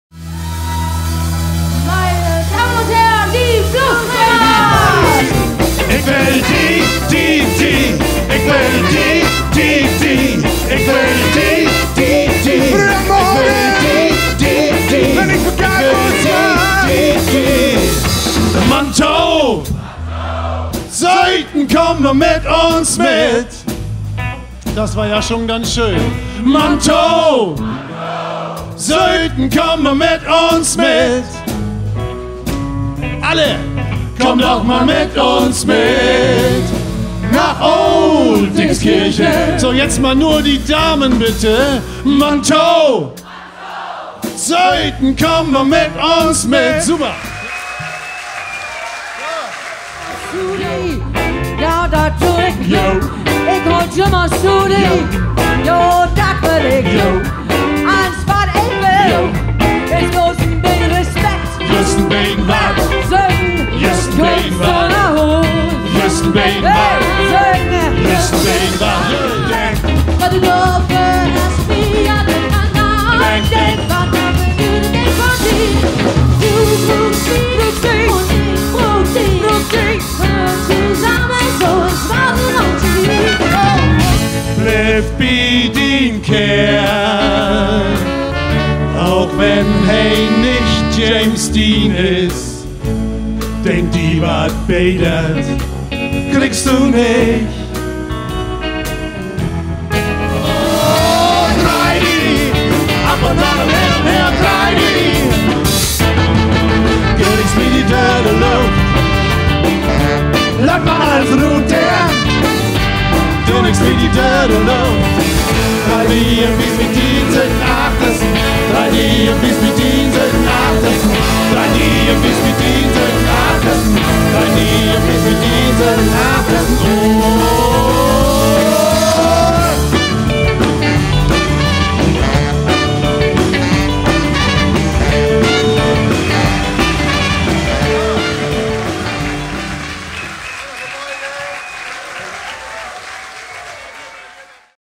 Bluus Bremers (2018)
Breminale 2018: Mehr als 200.000 Besucher lockte das Kulturfestival an den Bremer Osterdeich. Mit dabei waren auch die Bluus Bremers - sie singen auf plattdeutsch und zwar die Songs, die auch die Original Blues Brothers schon gesungen haben! Großartige Show, tolle Stimmung und mittendrin das Kamerateam der Winkler Studios!